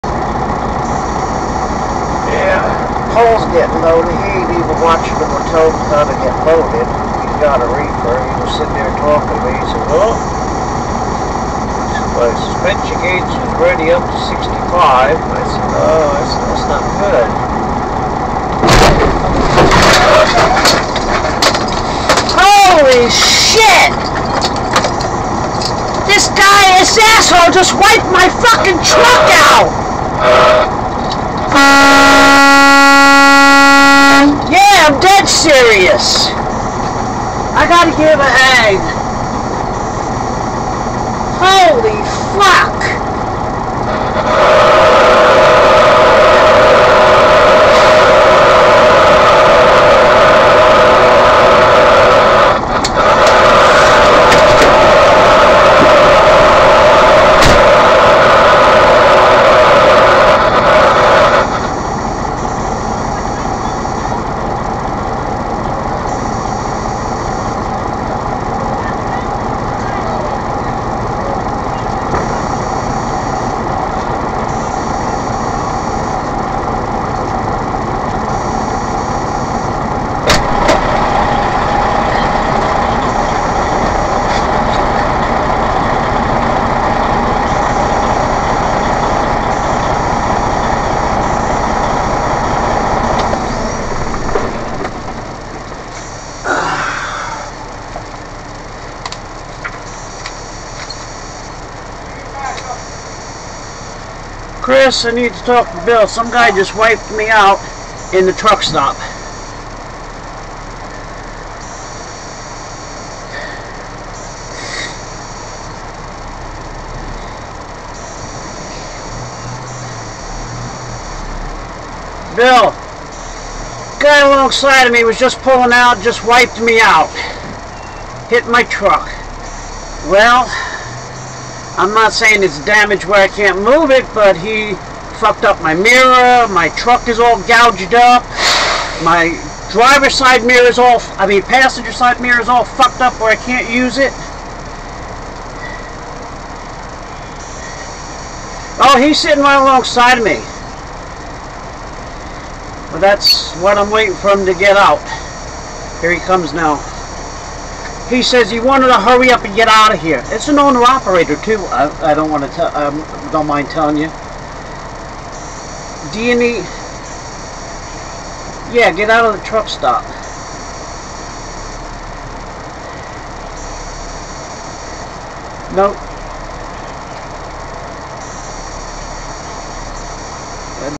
MOVI6523
accident  norman ok at the loves truck stop 8/30/17